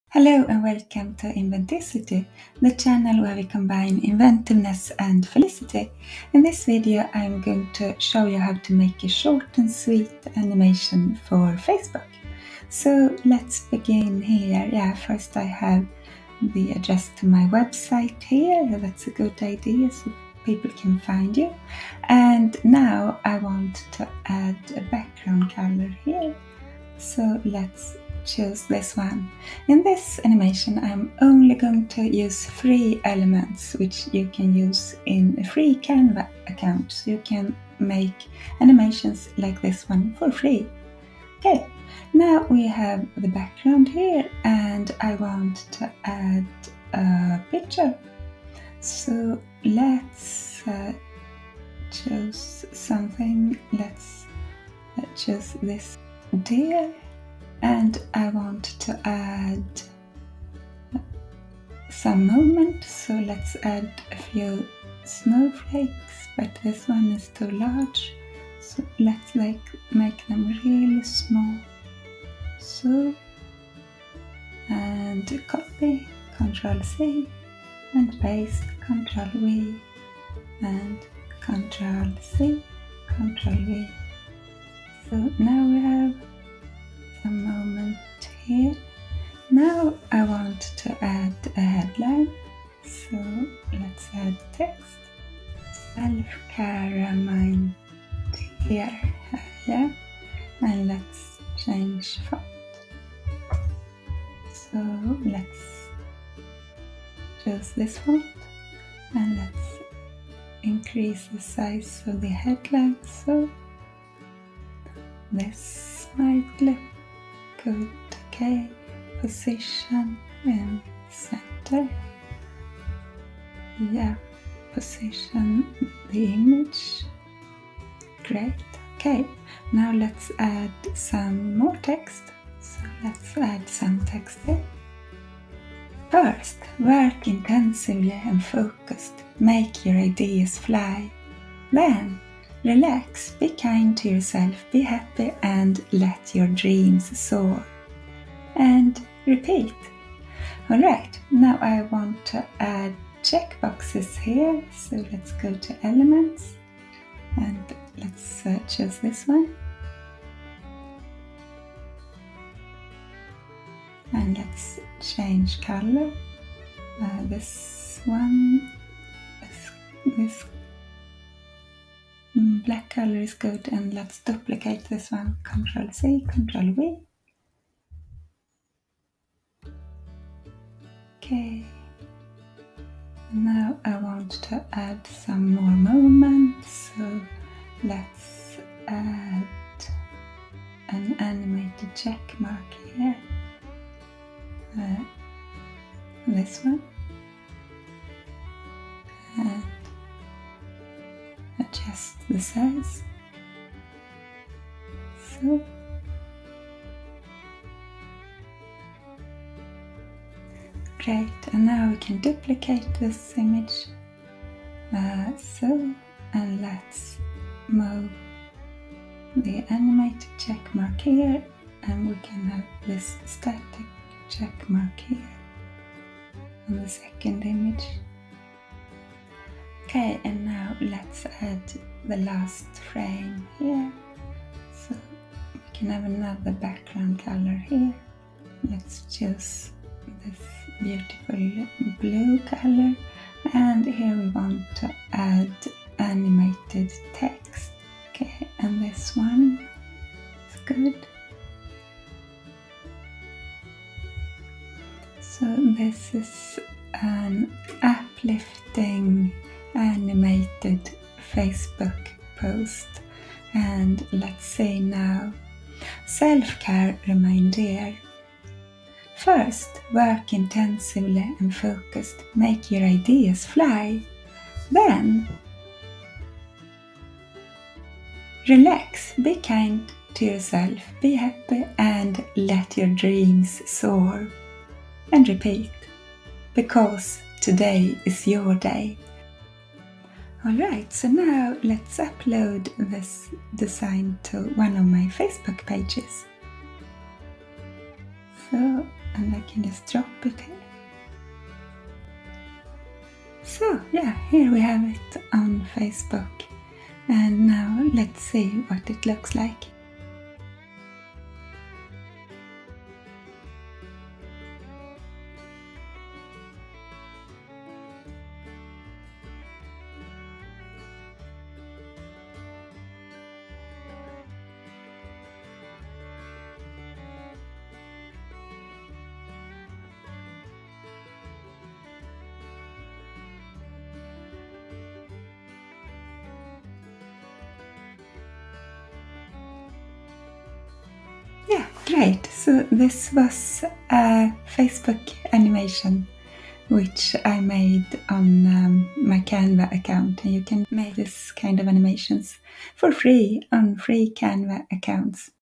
Canva Tutorial 2022 | How to Use Canva to Create Animated Social Media Posts for Free (Beginners)
Canva tutorial for beginners and pros. In this Canva tutorial, I'm going to show you how to use Canva to create animated posts for social media (for Facebook, Instagram, etc.). I'm only using free elements in this tutorial so you can make this design, and similar designs, for free with a free Canva account.  Create animated Facebook posts, Instagram posts,  Pinterest pins, etc. for free!

Cecilia